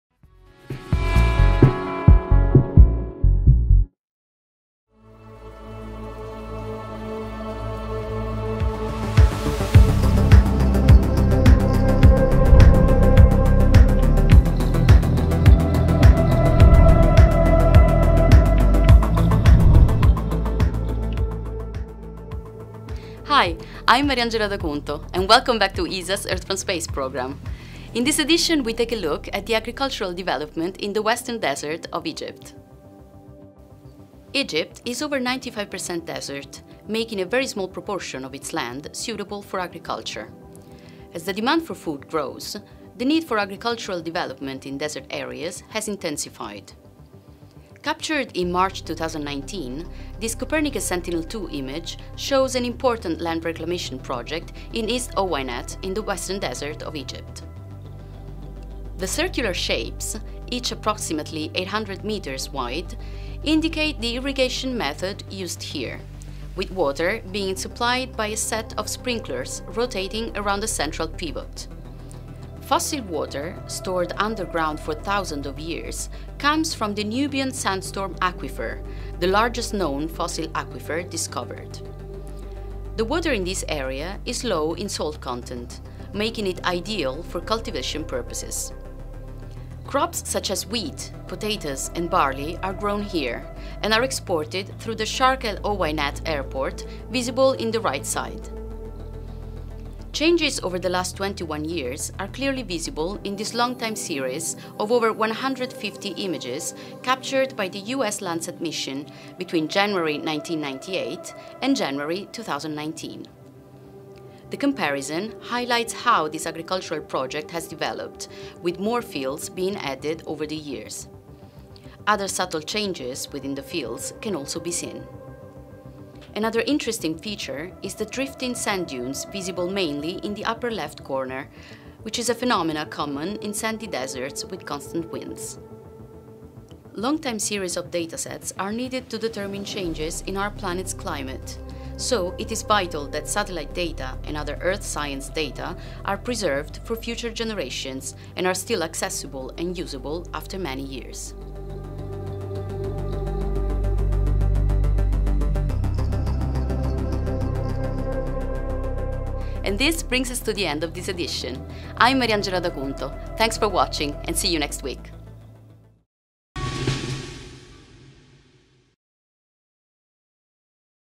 Watch Egyptian Crop Circles Form in Time-Lapse from Space
The US Landsat and Copernicus Sentinel-2 missions have captured imagery of the agricultural development in Egypt over time. See the crop circles form in this video from ESA.

Credit: ESA